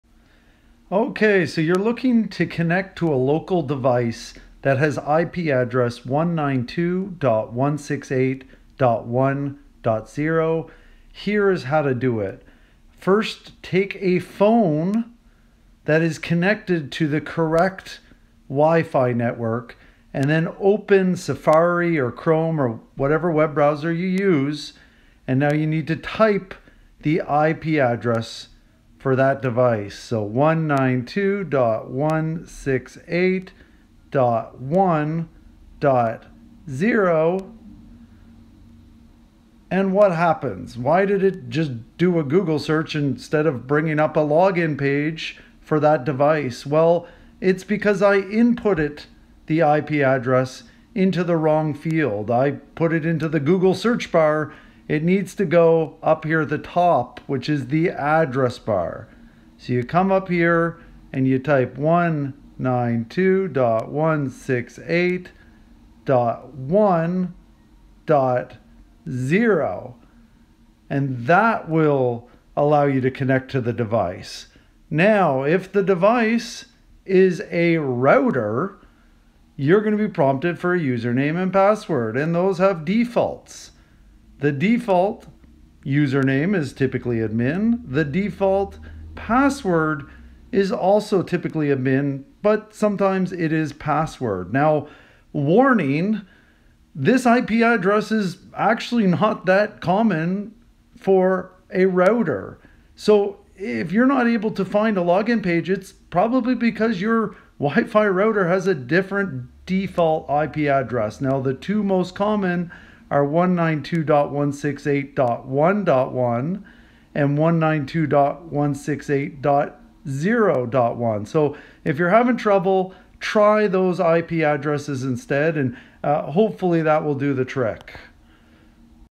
192.168.1.0 Admin Login Guide
Tutorial on how to connect to a local device, such as a Wi-Fi router, via admin IP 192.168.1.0 (a well-known private IP address for local devices such as routers).